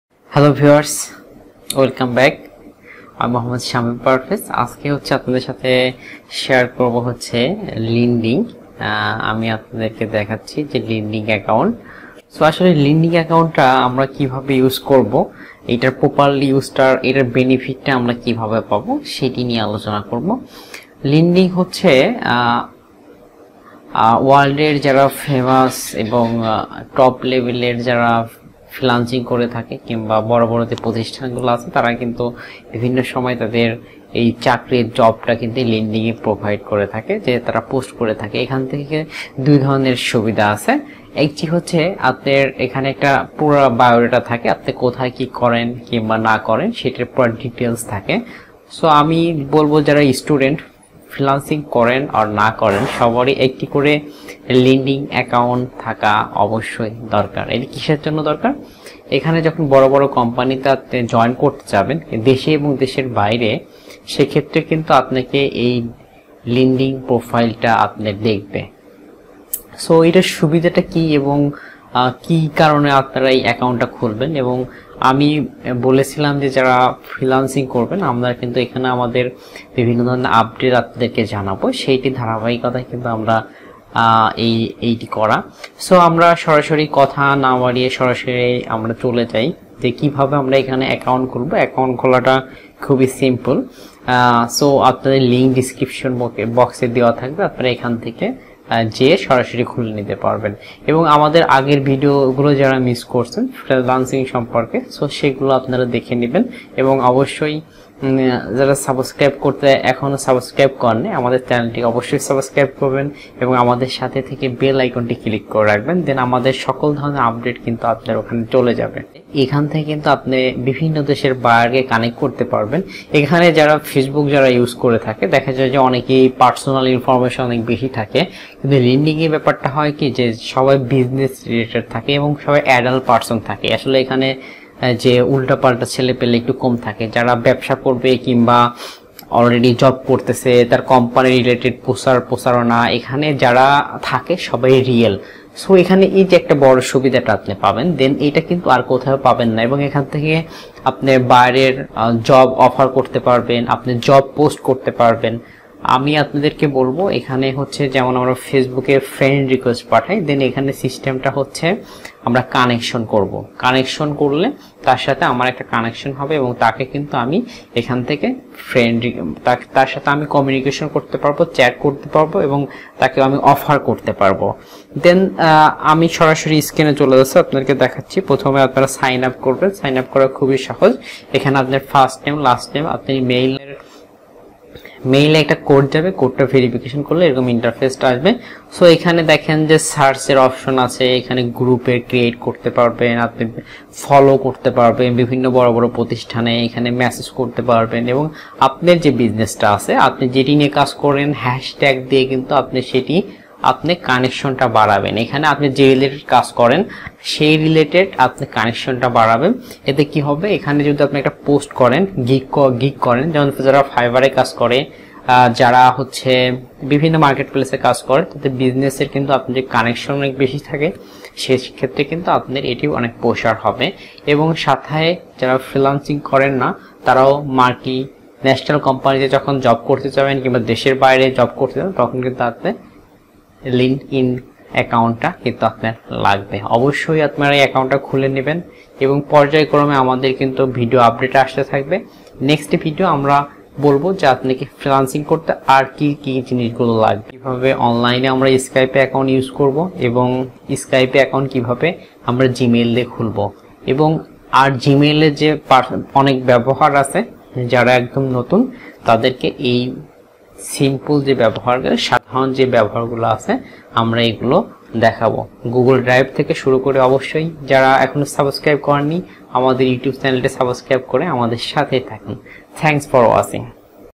online earning (ফ্রিল্যান্সিং) অনলাইনে কাজ শিখুন বিনামূল্যে | how to use LinkedIn account
অনলাইনে কাজ শিখুন বিনামূল্যে , সবাই বেশি বেশি শেয়ার করে সবাইকে শেখার সুযোগ করে দিন ।
how to use LinkedIn account

মাসে গড়ে ৬০-৭০ হাজার টাকা আয় হয়। কিভাবে ৬০-৭০ হাজার টাকা আয় হয় তার নিজ মুখে শুনুন